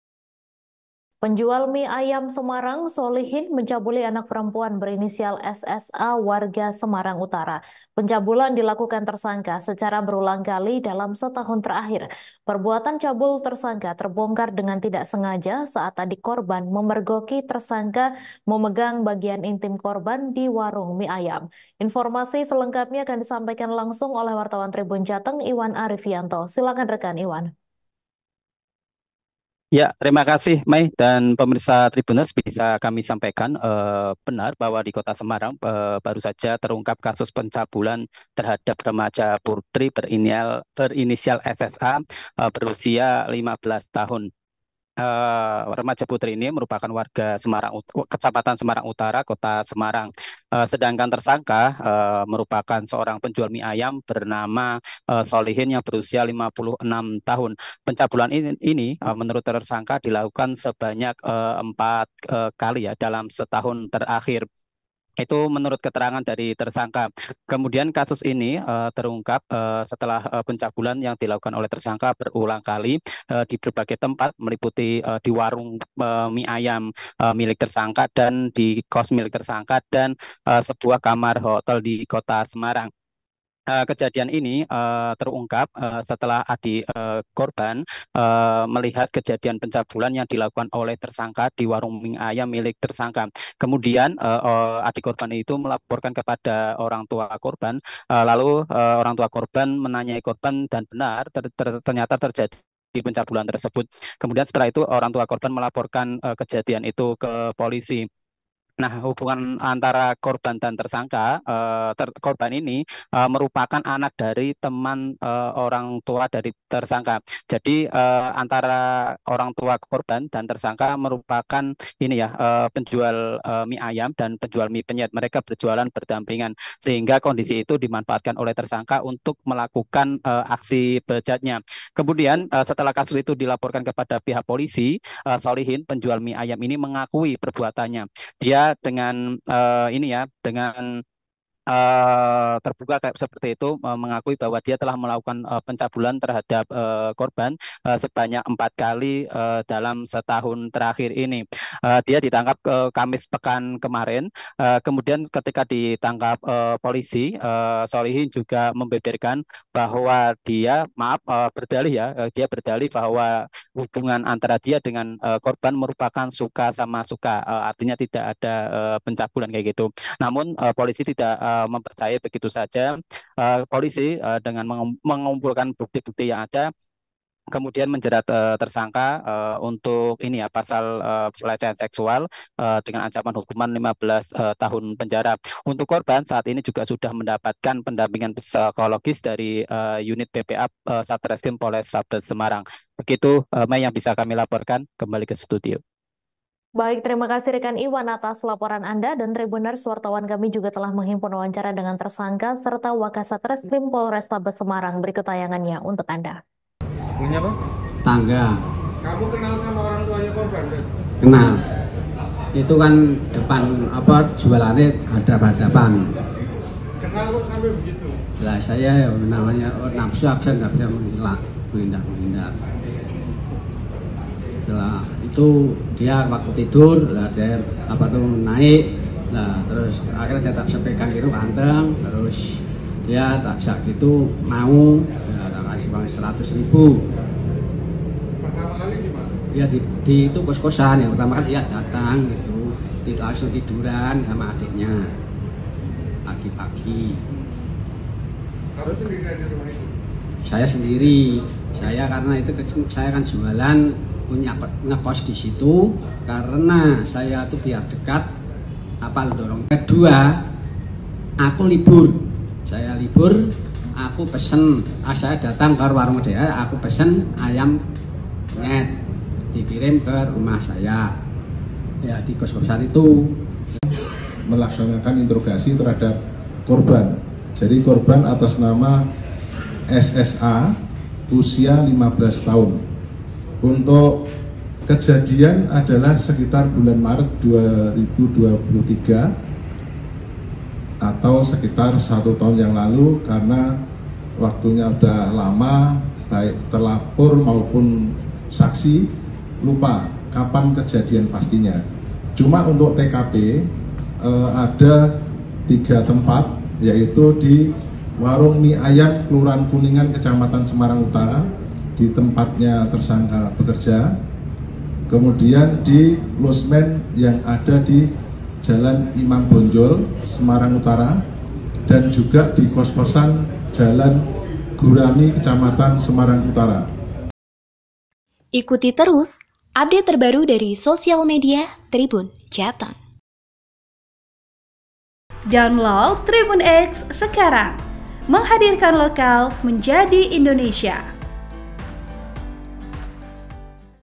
Penjual Mie Ayam Semarang Sholihin Cabuli Anak Penjual Ayam Penyet: Ketahuan Tak Sengaja oleh Adik
Penjual mie ayam Semarang, Sholihin (56) mencabuli anak perempuan berinisial SSA (15) warga Semarang Utara.